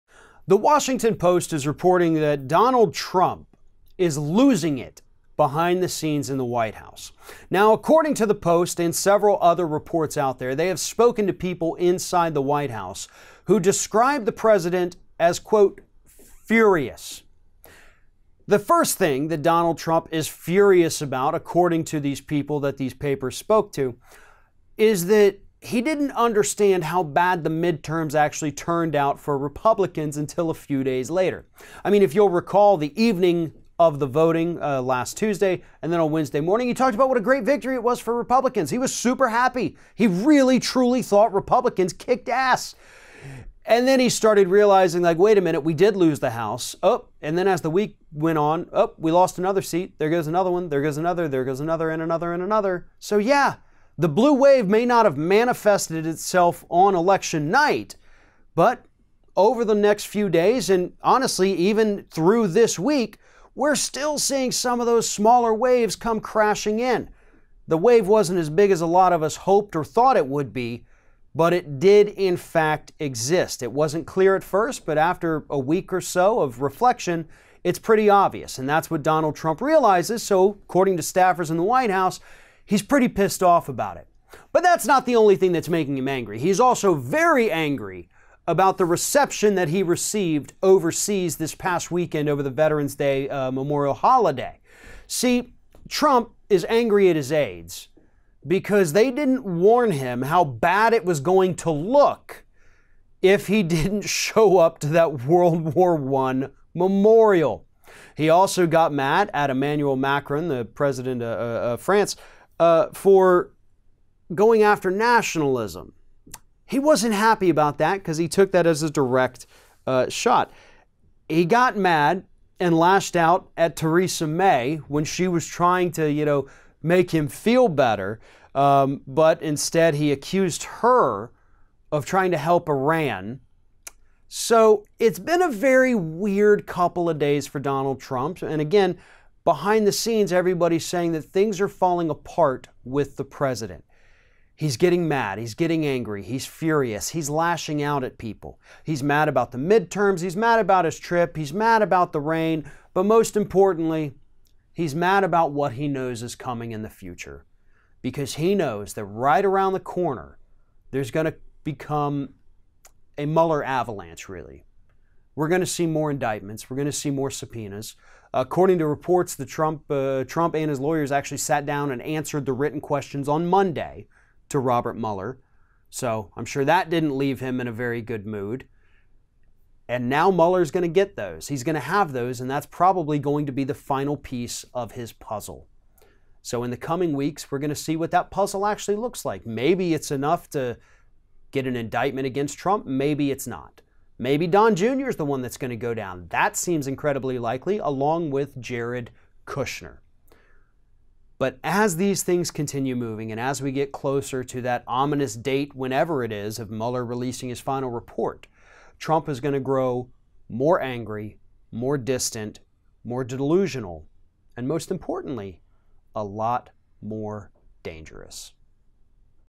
Trump Goes Off The Rails When He Realizes Republicans LOST The Midterms
Donald Trump seemed exceptionally pleased with the midterm results on the evening and morning after the voting took place. But things are not always as they seem, and behind the scenes Trump has been growing more and more angry about the results of the midterms now that he understands how badly the Republicans lost. He’s also fuming about his really bad trip over the weekend, and things aren’t looking up for him at this point. Ring of Fire’s Farron Cousins discusses this.